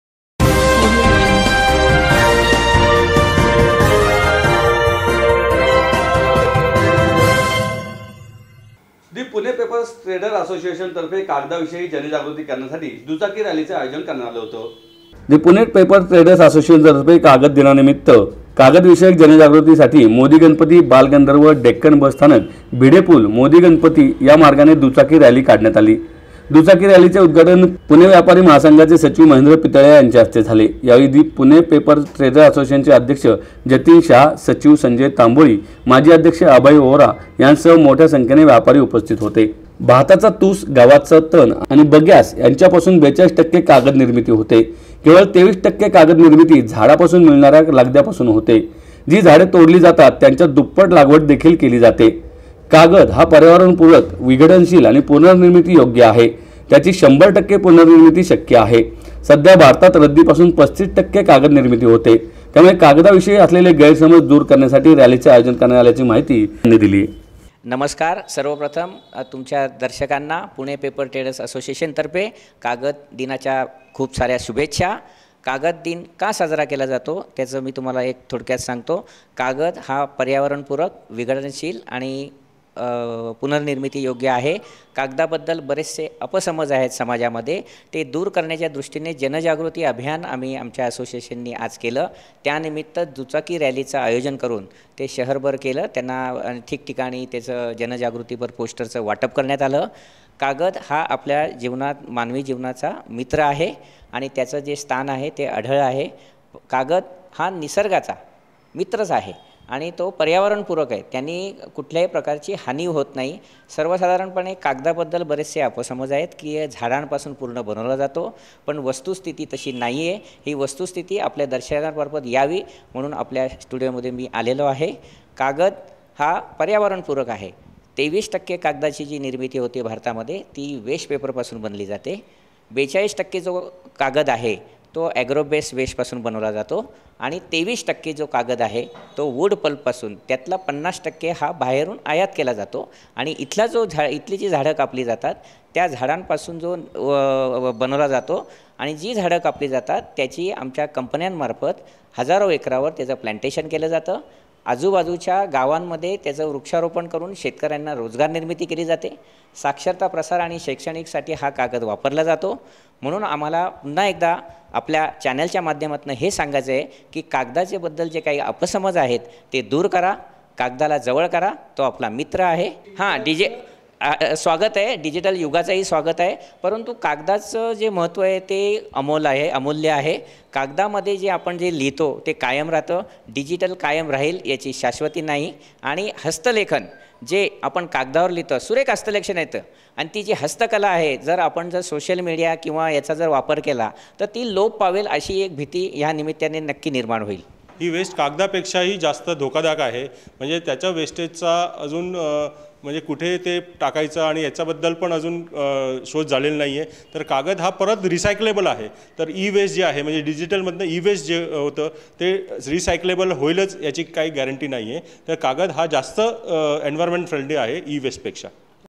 कागदविषयी गैरसमज नको...
दि पुणे पेपर ट्रेडर्स असोसिएशनतर्फे कागदाविषयी जनजागृती करण्यासाठी दुचाकी रॅलीचे आयोजन करण्यात आले होते.